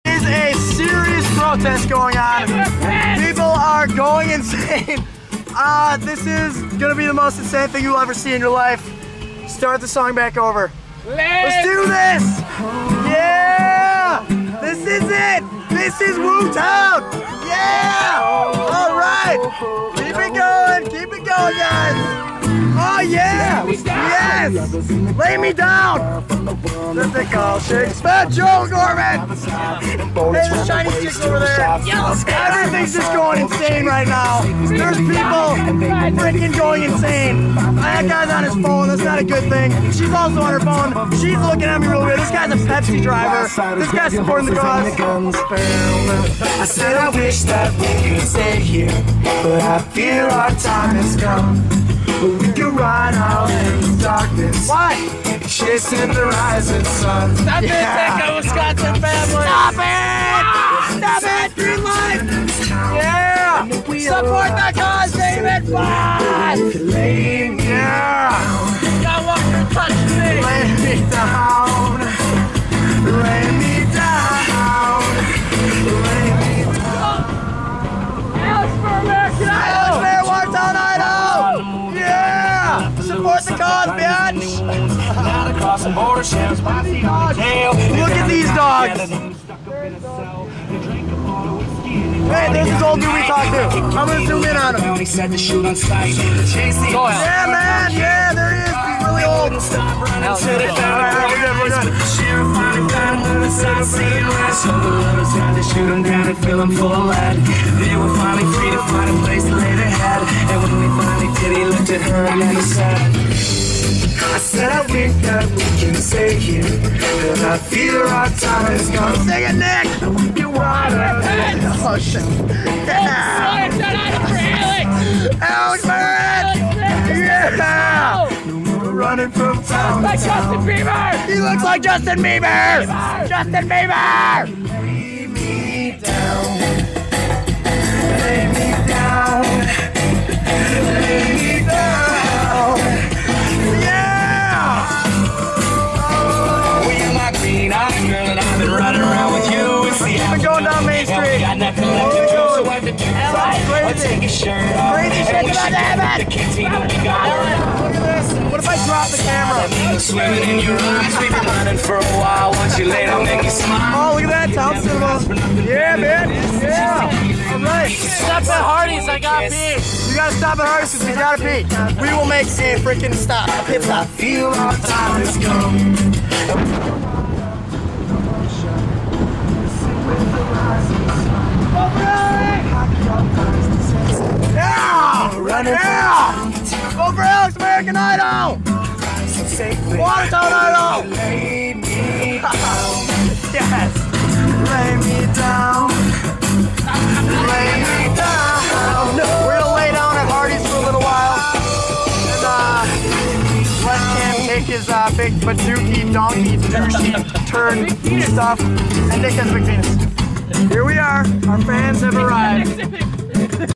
CRAZIEST DAY EVER! Student protest at Watertown, WI
Alex Merritt, Nick Sanford, and Cam Cudentaffordahouseki going insane down main street seeing protesters gone wild!